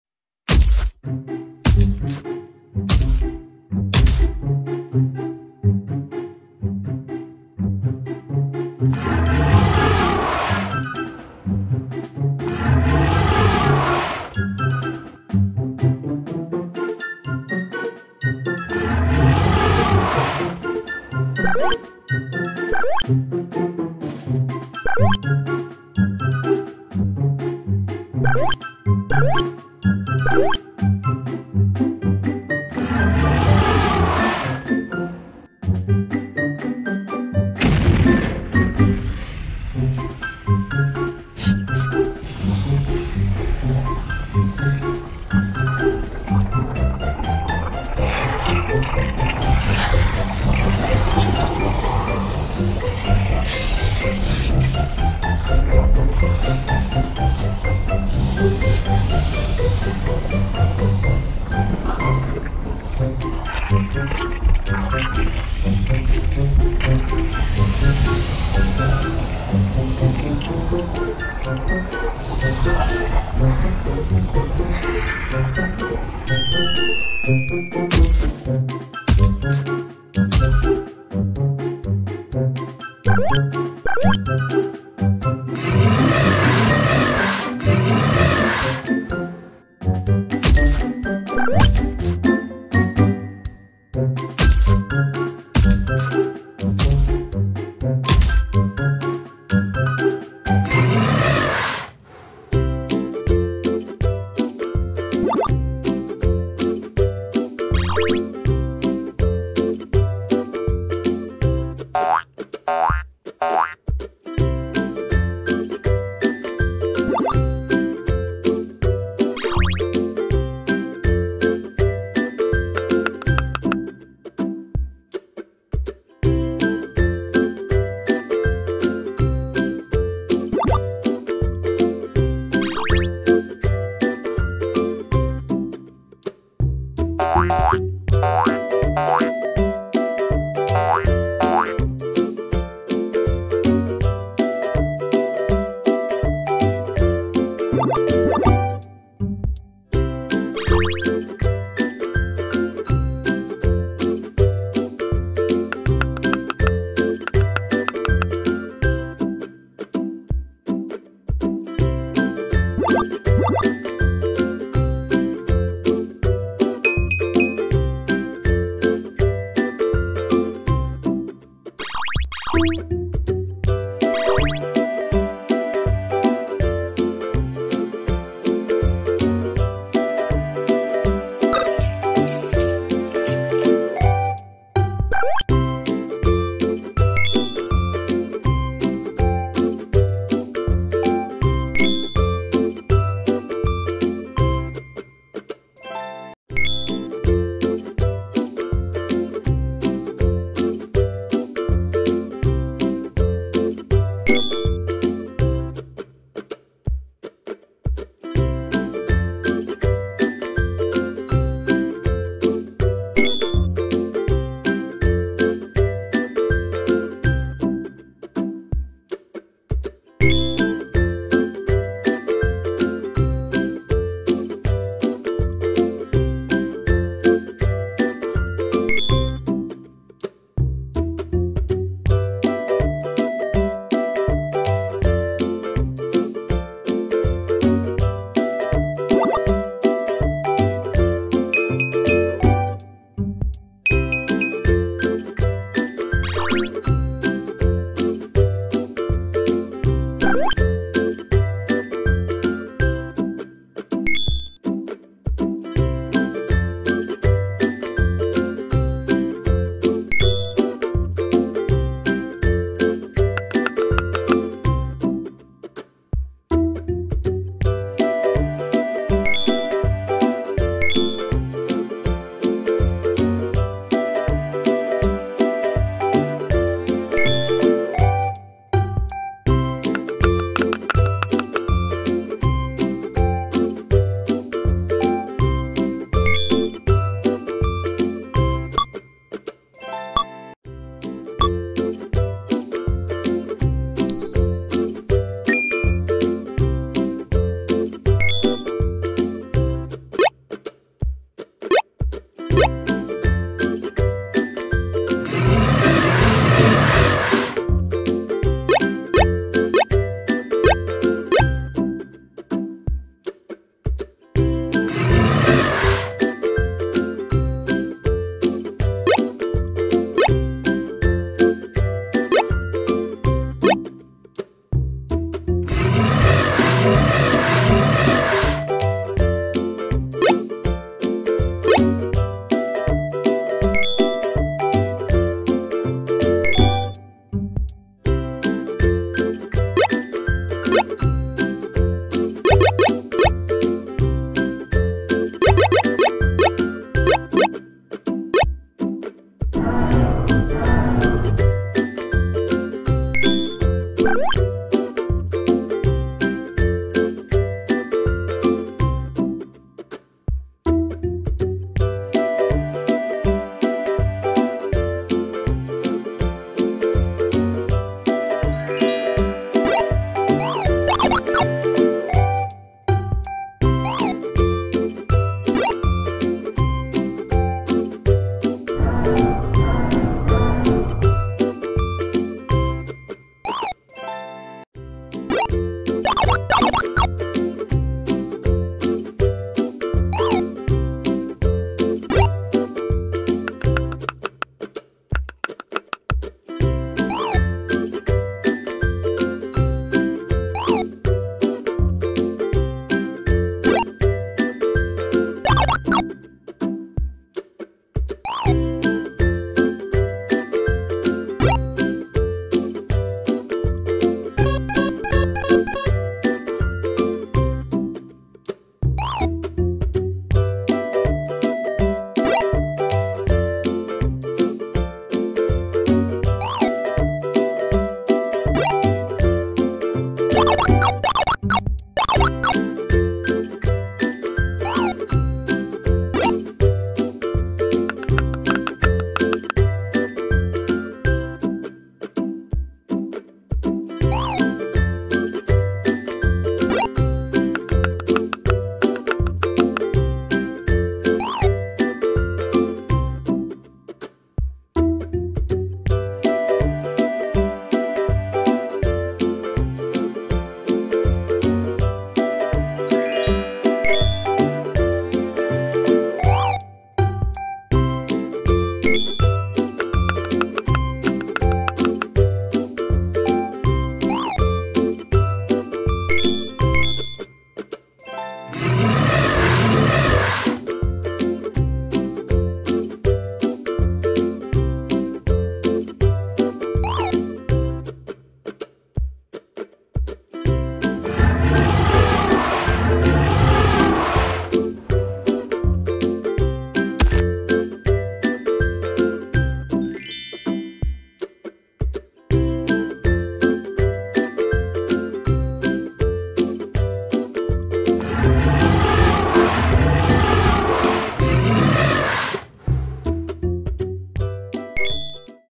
Tyrannosaurus Rex | Dinosaur Eggs | Volcanic eruption
T-Rex watches the eruption of a volcano, after which dinosaur eggs appear. You can see the family of Veloceraptor, stegosaurus, stygimolochus, triceratops and 4 tyrannosaurs, including the dominant t-rex